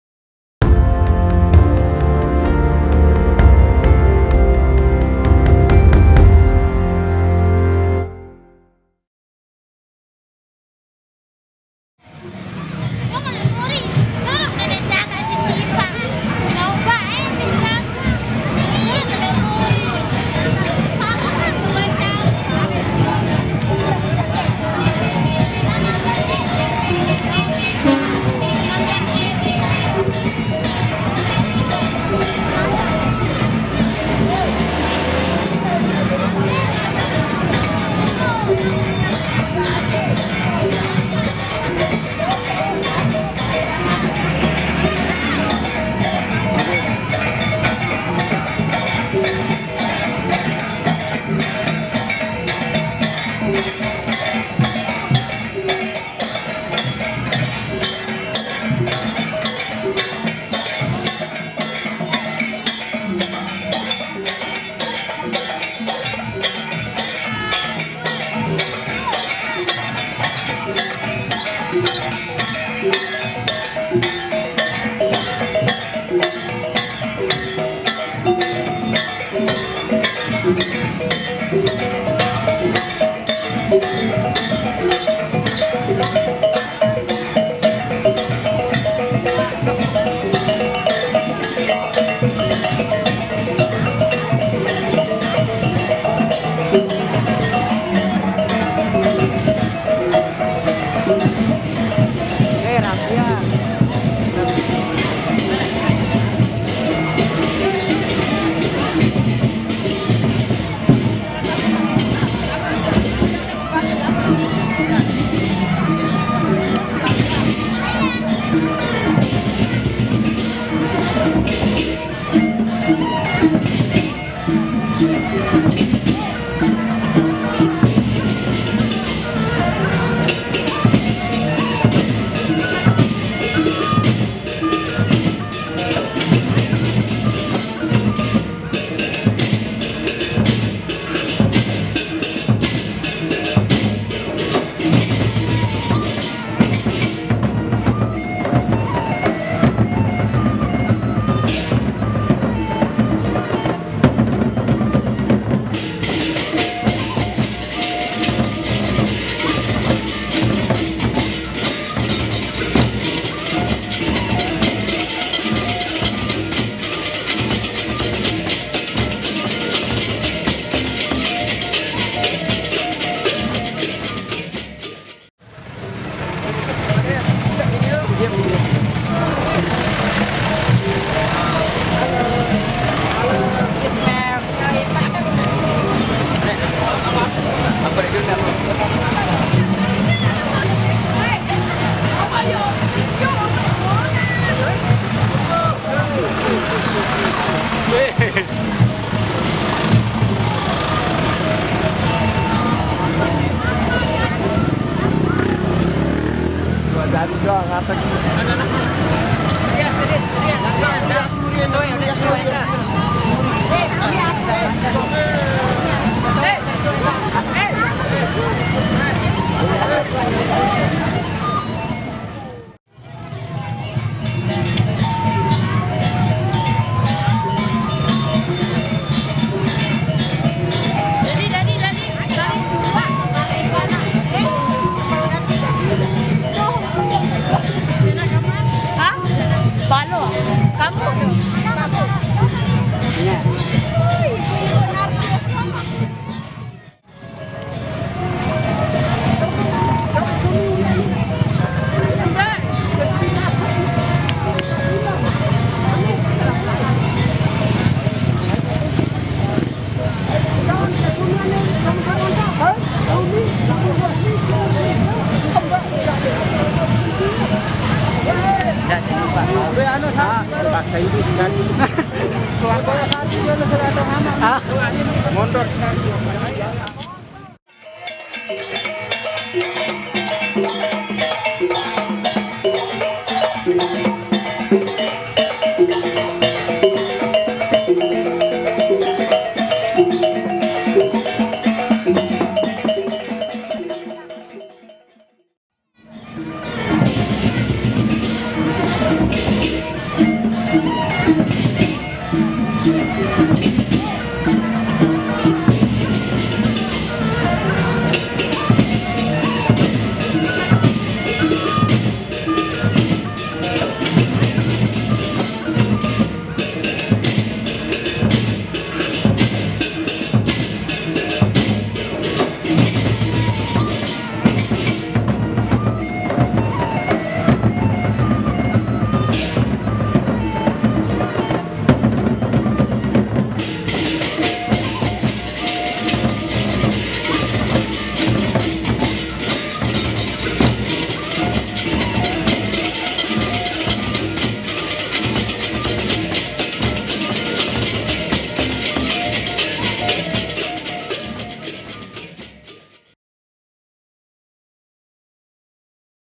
persatuan banjar leong Jaya(PBLJ) || acara nyonkolan semeton banjar heri dan dina
nyonkolan adalah tradisi adat lombok yang sudah di tetapkan dari dulu, nah disini adalah acara nyonkolan kami dimana ia adalah salah satu anggota banjar dari PBLJ(persatuan banjar leong jaya) selamat menonton, jika suka dengan videonya jangan lupa like,komen dan subscribe yah,,,,